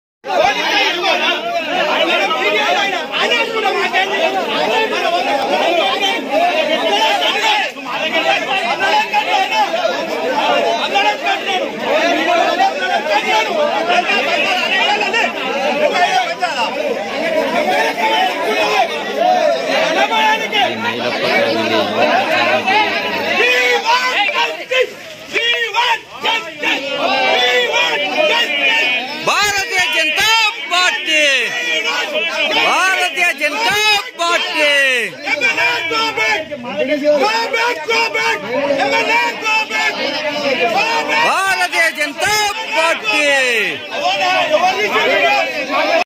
మునుగోడులో టీఆరెస్ ఎమ్మెల్యేపై నిరసన గళం..వెనక్కి వెల్లలంటూ వాగ్వాదం..బీజేపీకార్యకర్తలపై గుండాల దాడి
మునుగోడులో టీఆరెస్ ఎమ్మెల్యేపై నిరసన గళం.. వెనక్కి వెల్లలంటూ వాగ్వాదం... బీజేపీ VS టీఆరెస్ లొల్లి